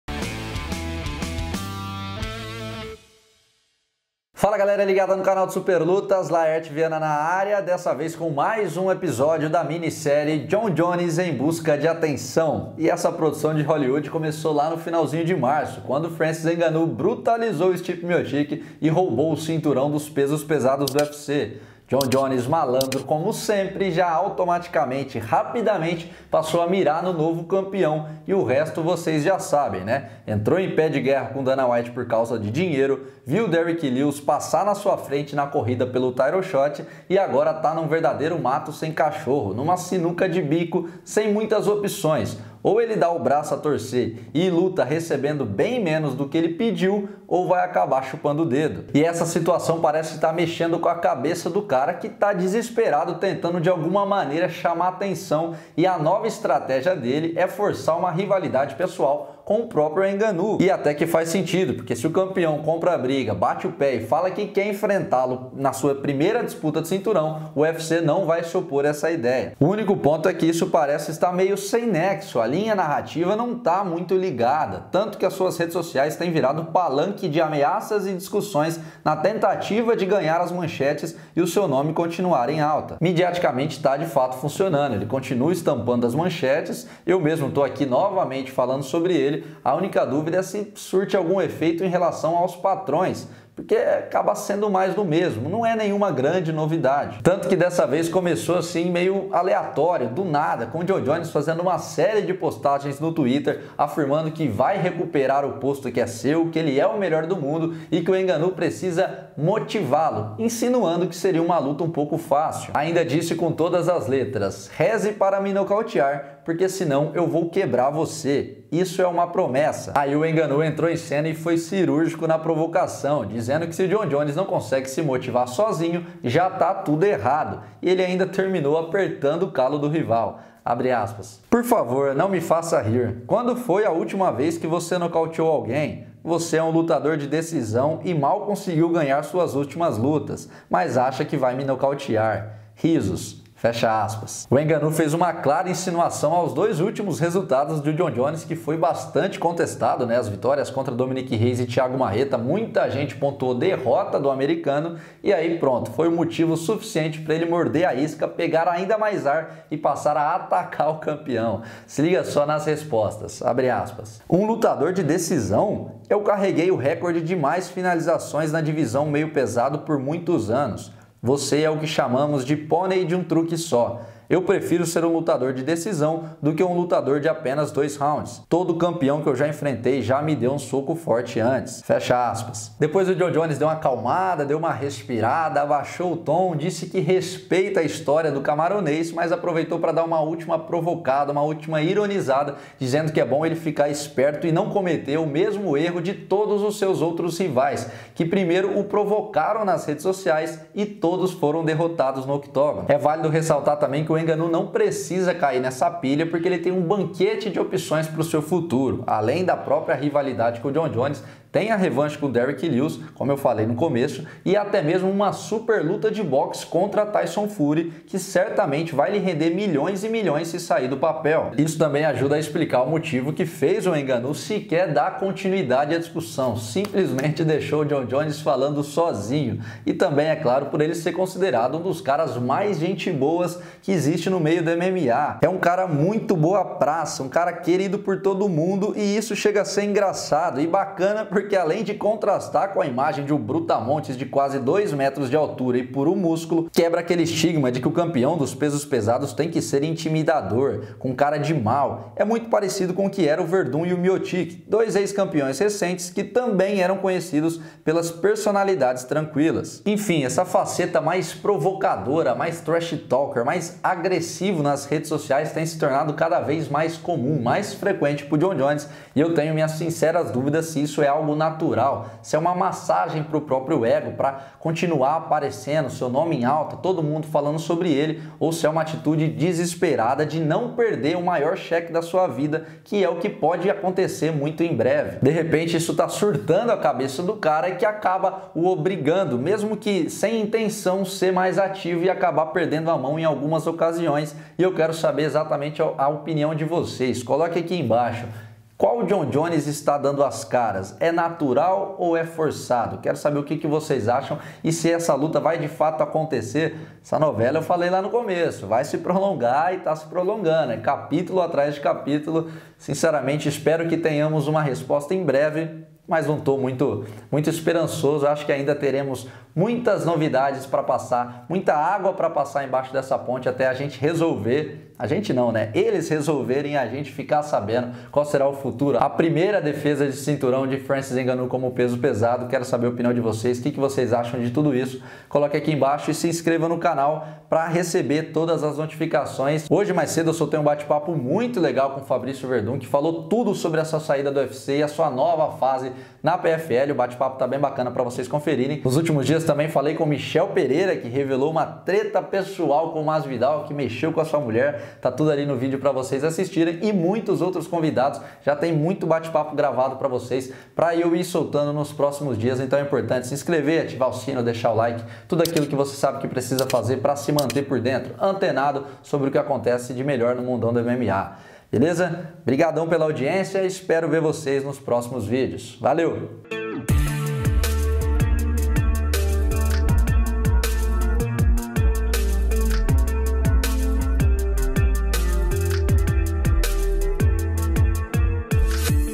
JON JONES E NGANNOU TROCAM AMEAÇAS E CLIMA ESQUENTA
Francis Ngannou não deixou barato e respondeu as provocações. Confira tudo!